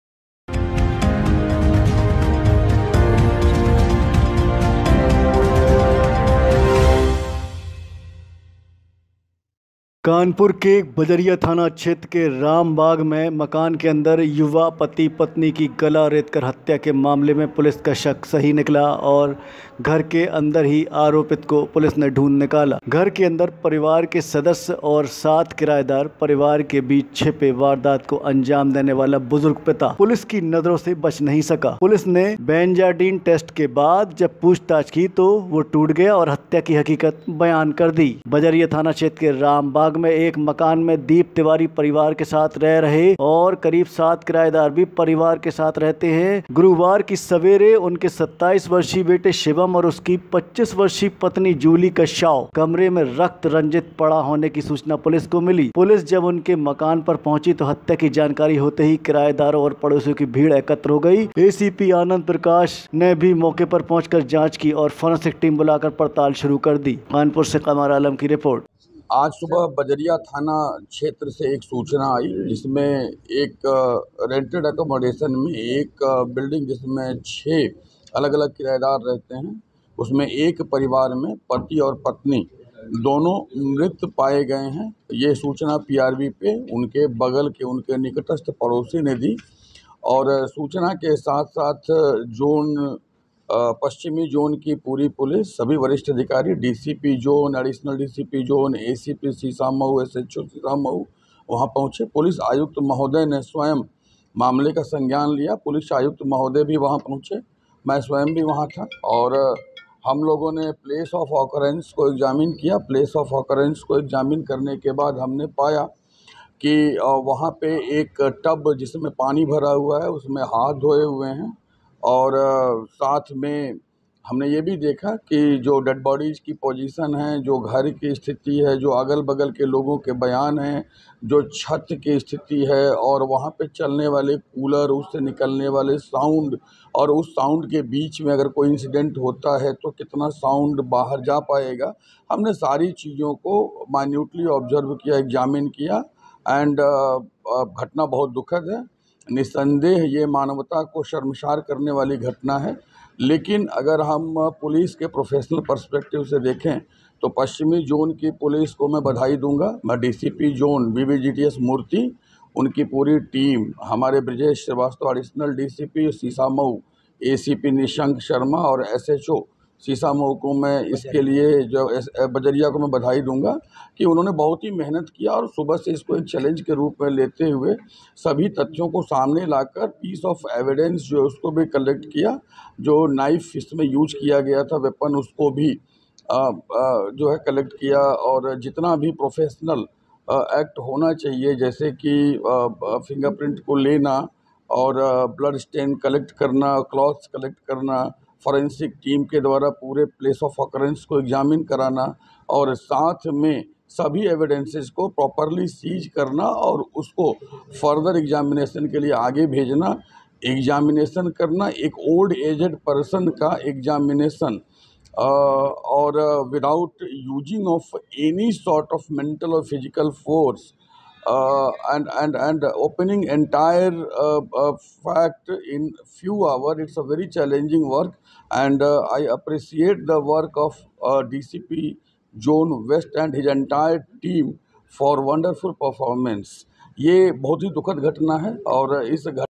कानपुर: सही निकला पुलिस का शक, बुजुर्ग पिता ने उतारा था बहू-बेटे को मौत के घाट
बजरिया थाना क्षेत्र के रामबाग में घर के अंदर युवा पति-पत्नी की गला रेत कर हत्या के मामले में पुलिस का शक सही निकला और घर के अंदर ही आरोपित को ढूंढ निकाला।